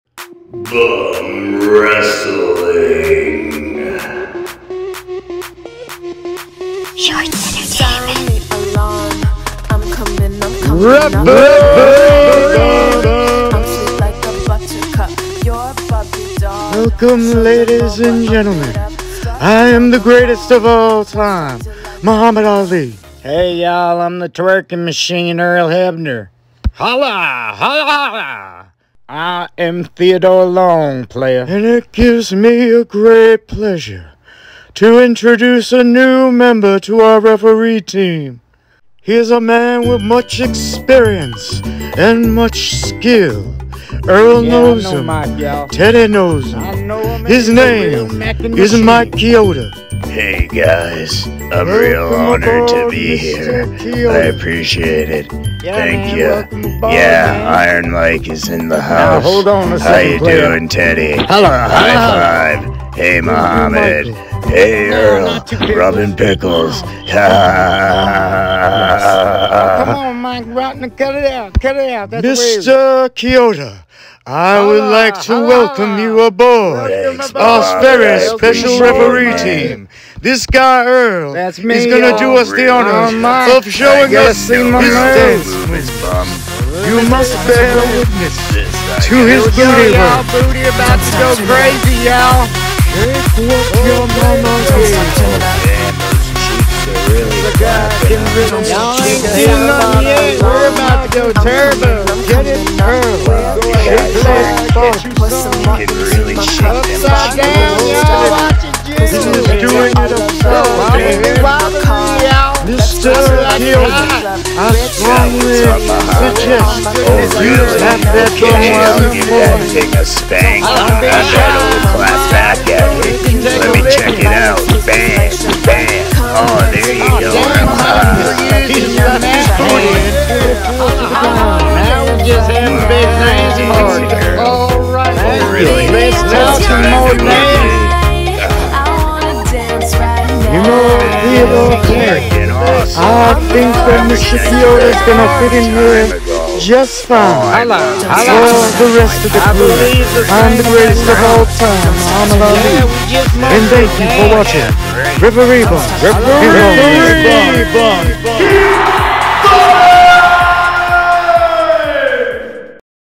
Referee Bumps BUM WRESTLING FANT FIG FED Show with Muhammad Ali Welcoming a New Ref to the Team 2024
Referee Bumps
BUM WRESTLING FANT FIG FED 2024
with Muhammad Ali, Earl Hebner & Teddy Long
Welcoming a New 4th Ref to the Team!
9/9/24

Musician: Glow Stick